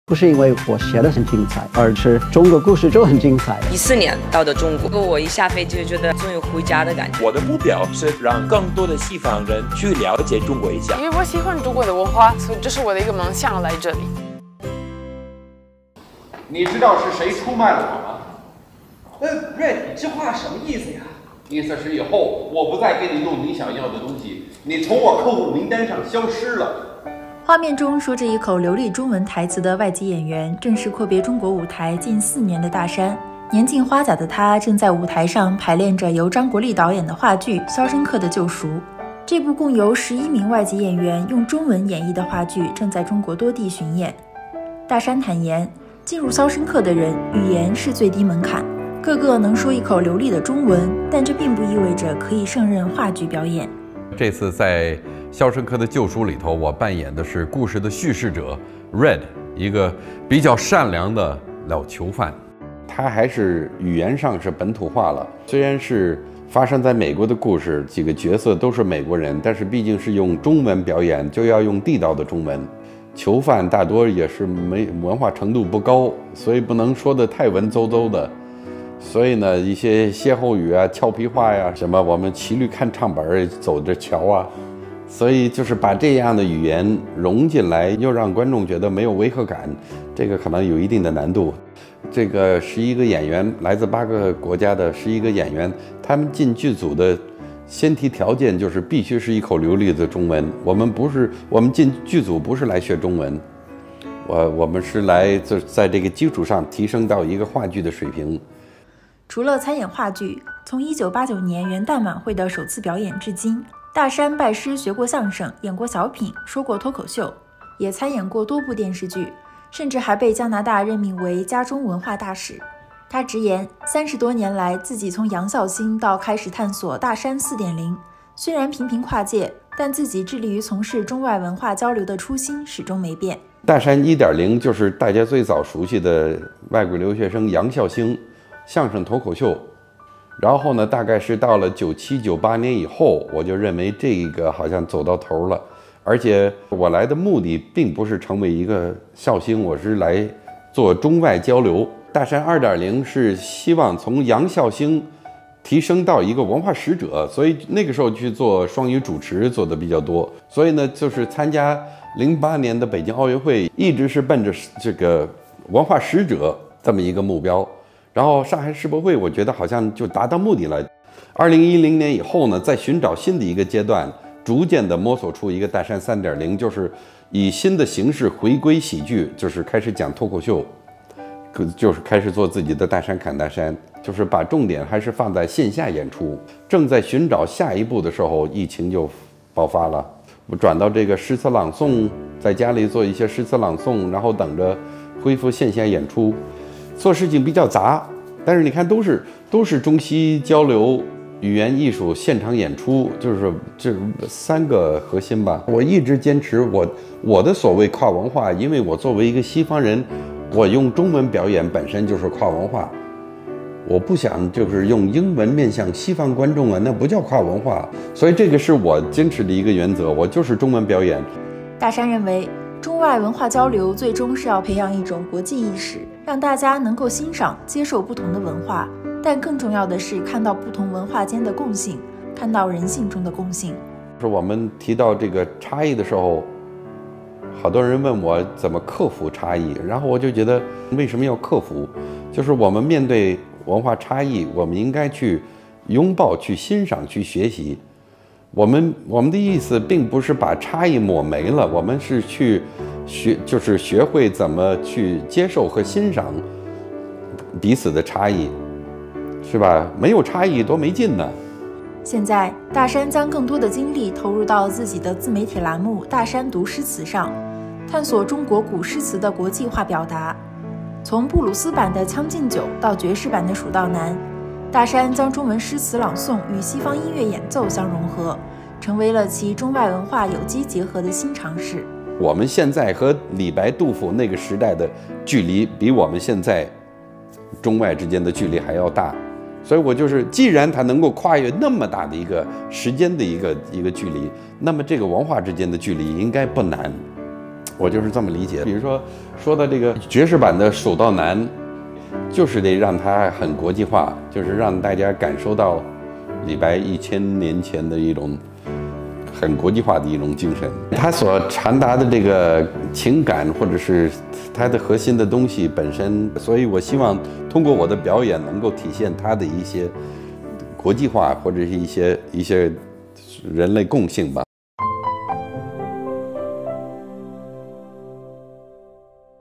【洋腔队】阔别中国近四年 演员大山讲述“洋笑星”跨界之路
画面中说着一口流利中文台词的外籍演员正是阔别中国舞台近4年的大山，年近花甲的他正在舞台上排练着由张国立导演的话剧《肖申克的救赎》，这也是大山时隔近4年再度回到中国后接下的第一个线下演出项目。这部共由11名外籍演员用中文演绎的话剧正在中国多地巡演。
除了参演话剧，从1989年元旦晚会的首次表演至今，大山拜师学过相声、演过小品、说过脱口秀、也主演过多部电视剧，甚至还被加拿大任命为“加中文化大使”。他直言，30多年来，自己从“洋笑星”到开始探索“大山4.0”，虽然频频跨界，但自己致力于从事中外文化交流的初心始终没变。（李佳励 官逸伦）